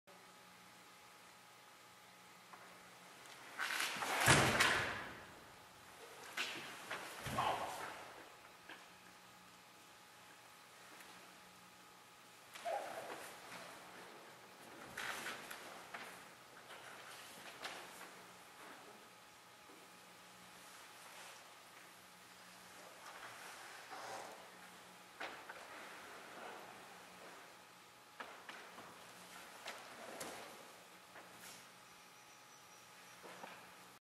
30 seconds iaido
Doing a little Iaido practice at Gokurakuji, Japan.